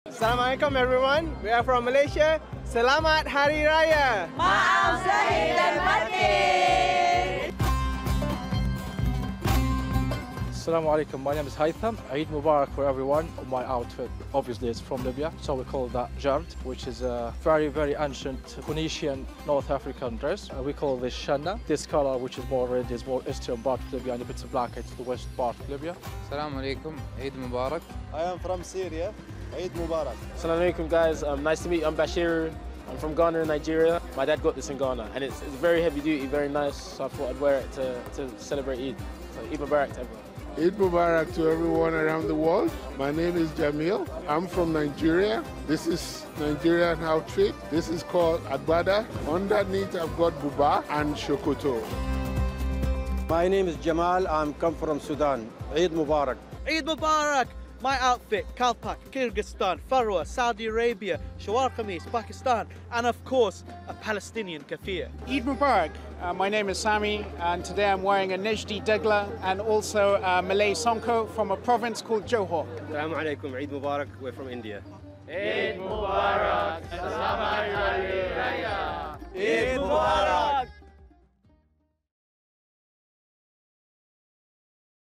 Eid in London through diverse cultural outfits
TRT World captures the spirit of a multicultural Eid al-Fitr in central London, as people wear traditional, cultural outfits from around the world.